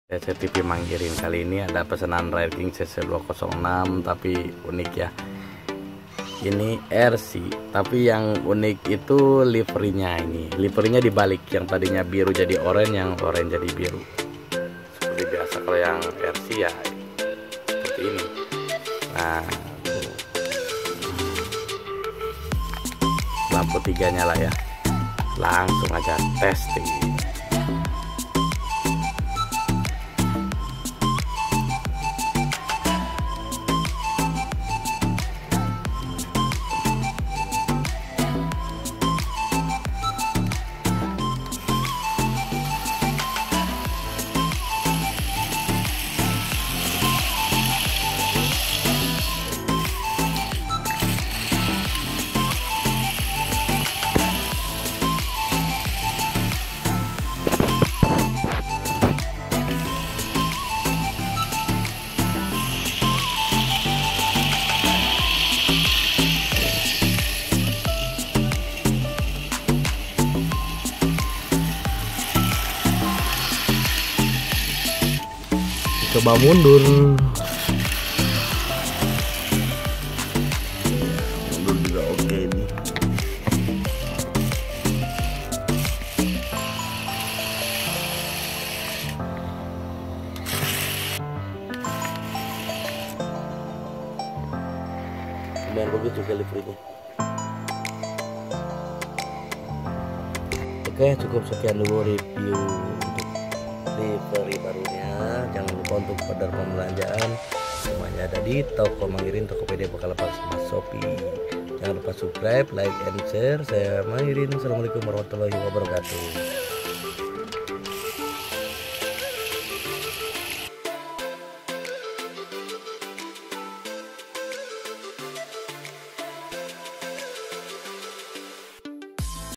miniatur cc206 RC remote control livery terbalik
SULTAN MAH BEBAS
livery ini merupakan request dari yg beli dan alhamdulillah bisa direalisasikan dalam miniatur, desain sama hanya beda warna di livery

jenis pengoprasian  RC ( Remote Control )
skala HO 1/87 Railking